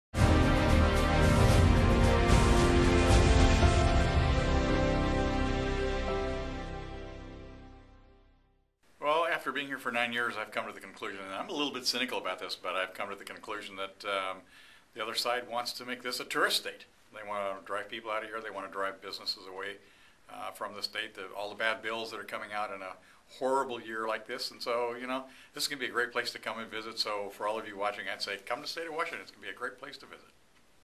GOPod VIDEO: Sen. Mike Hewitt on Washington Becoming a Tourist State
16th District Senator and Republican Leader Mike Hewitt says welcome to Washington State!  It's a great place to visit, but you won't want to do business or live here...